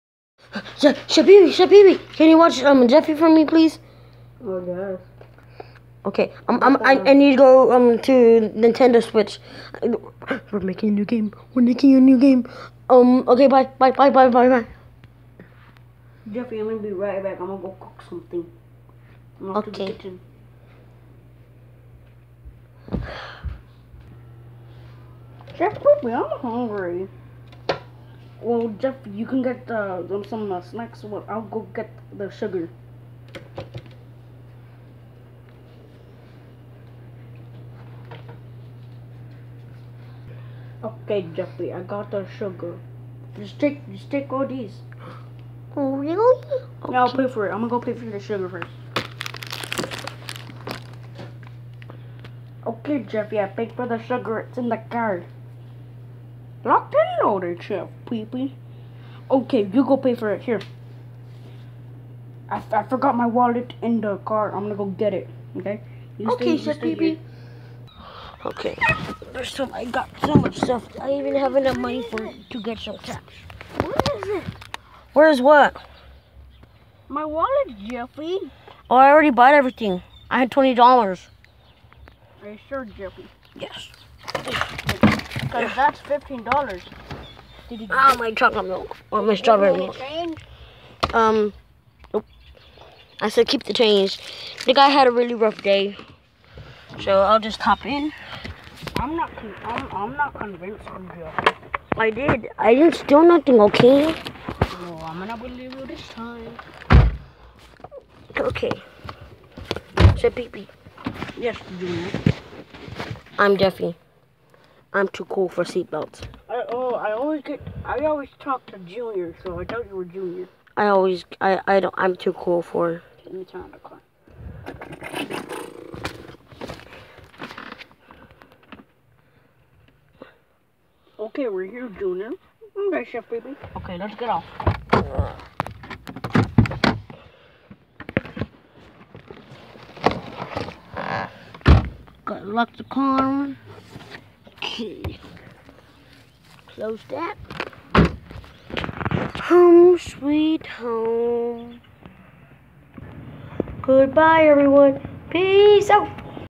CHEFF PEEPEE WATHES JEFFY
today chefs peepee watches Jeffy continue waiting to see what mysterious things jeffy has up his DIAPER! ;);););););)